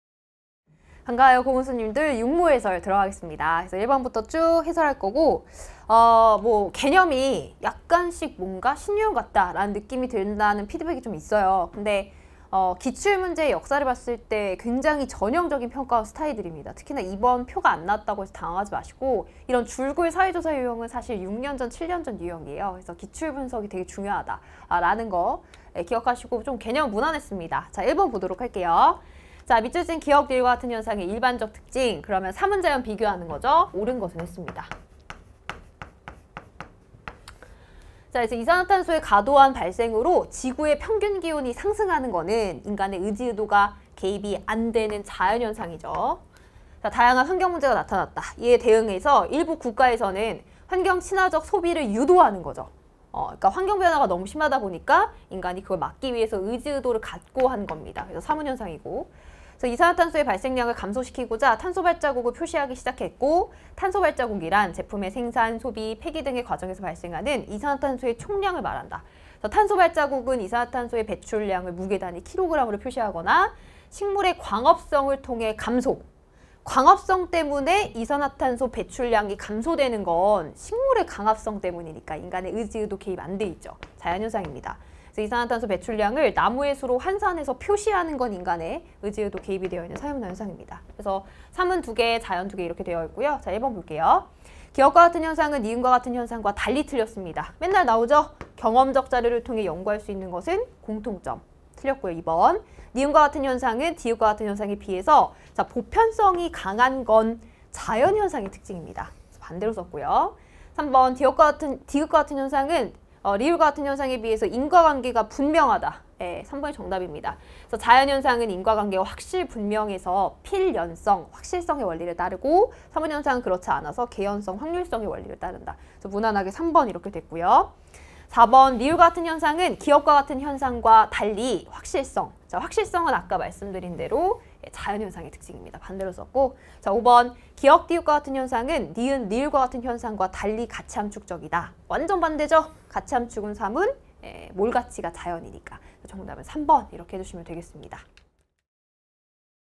[메가스터디] 사회문화 손고운 쌤 - 2022학년도 6평 사회문화 해설강의
사회문화 손고운 선생님의
2022학년도 고3 6월 모의평가 사회문화 해설입니다.

* 더 많은 해설이 궁금하다면?

* 손고운 선생님에 대해 더 알고싶다면?